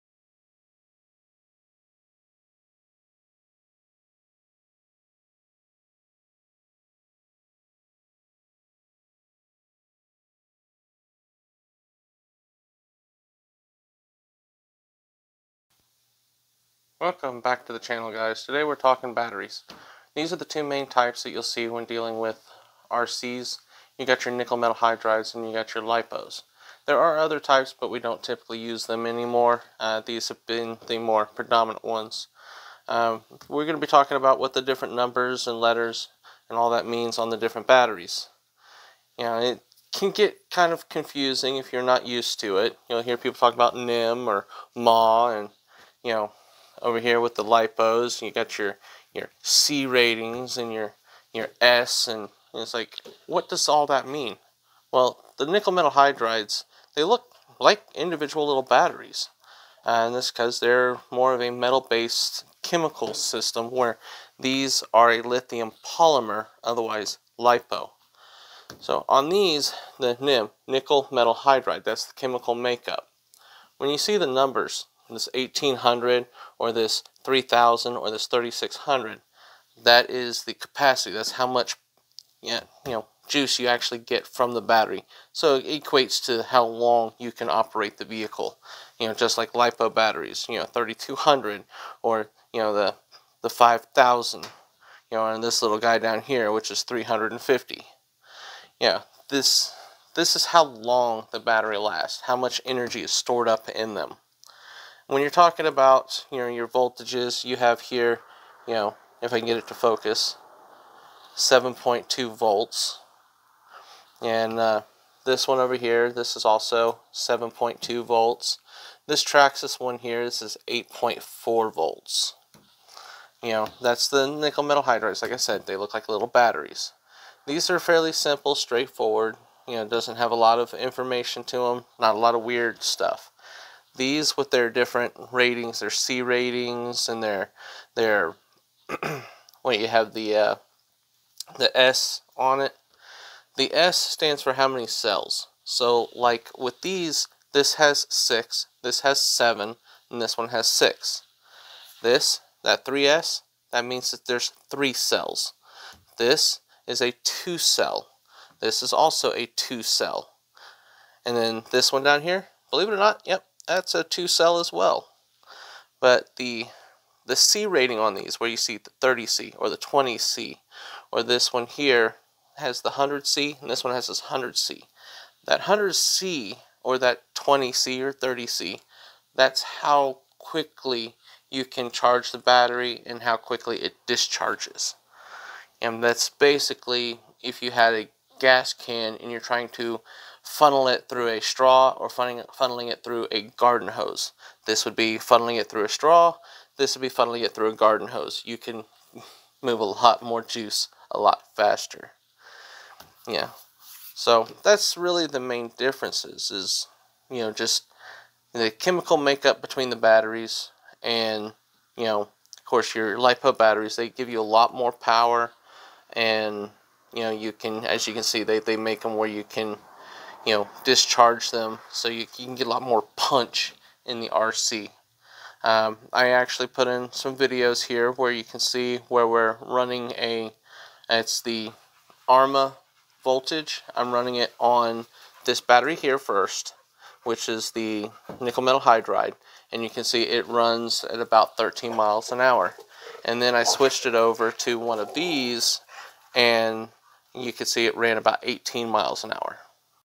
Basics of batteries for beginners
Helping you know the basics and understand the strengths and differences between the types of batteries for your RC.